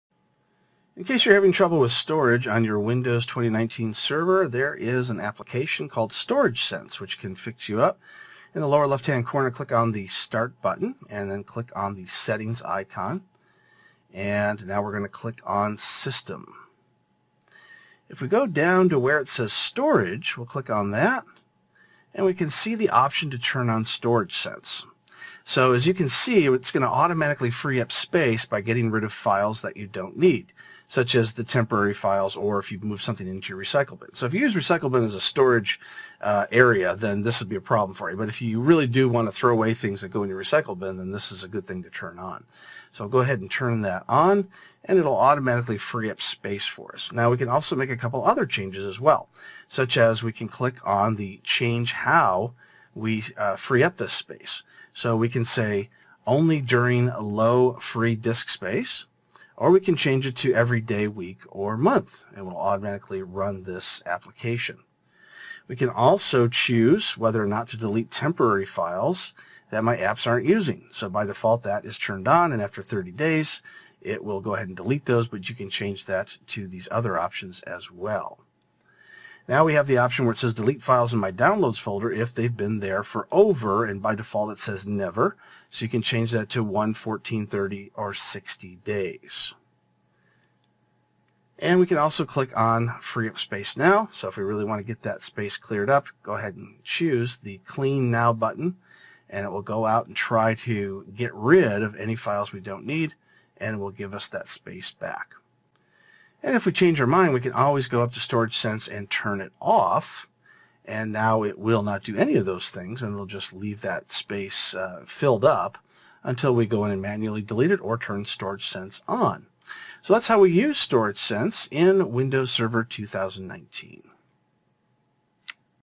Windows Server 2019 How to use Storage Sense
Professor Robert McMillen shows you how to use Storage Sense in Windows Server 2019 to keep your hard drive cleaned up to free up space automatically.